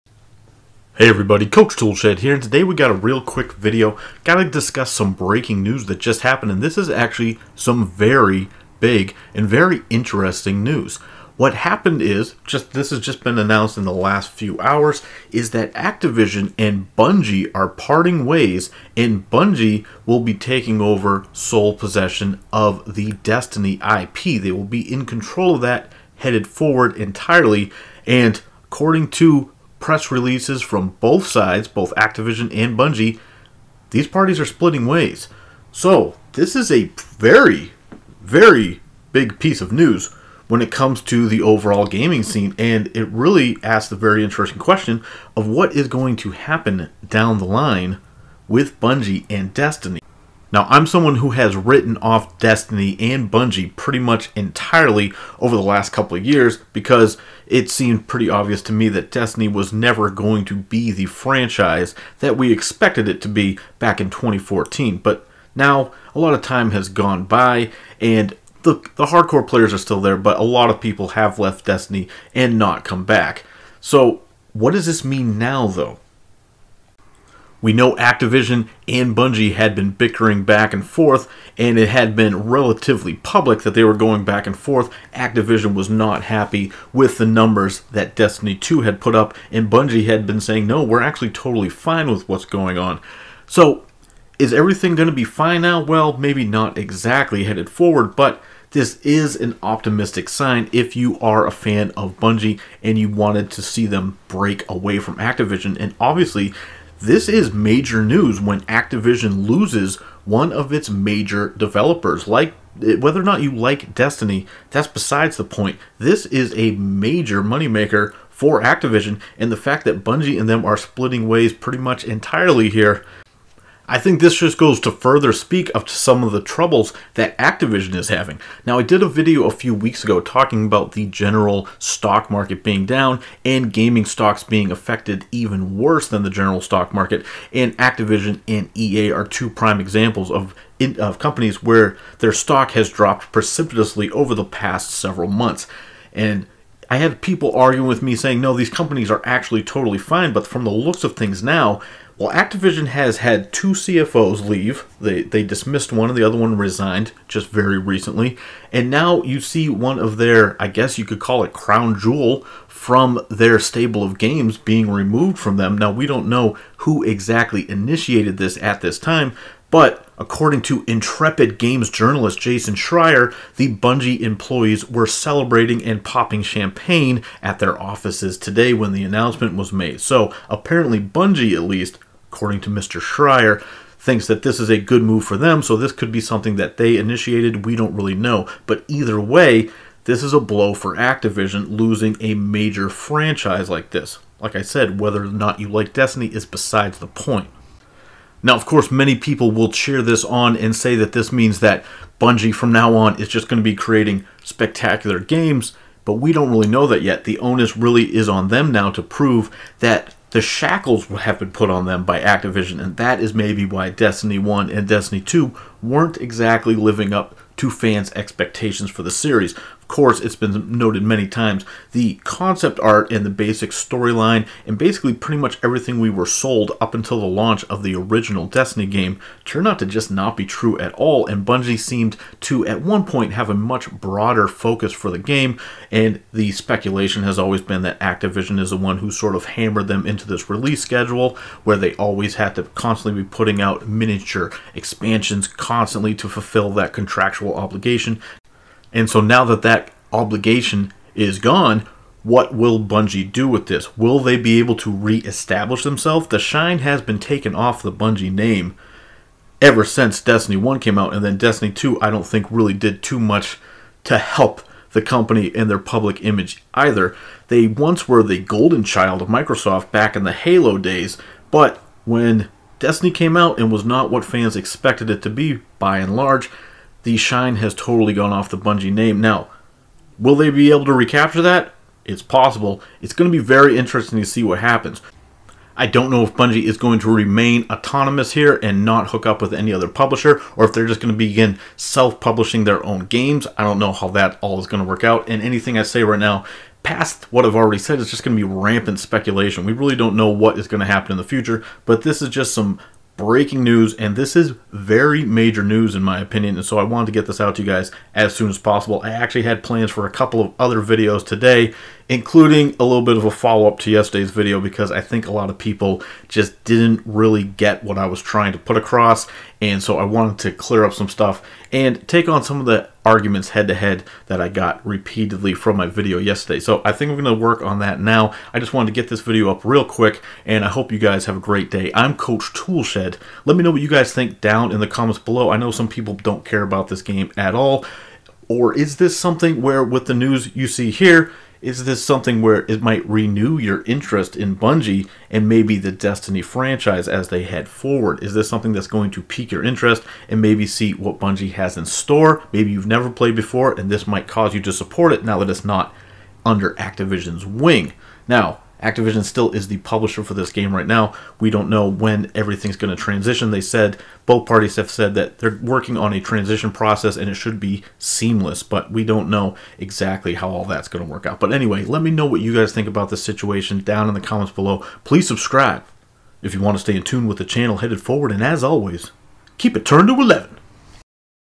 Bungie Dumps Activision, Keeps Destiny
Huge news just broke regarding Bungie and Activision.   Coach talks...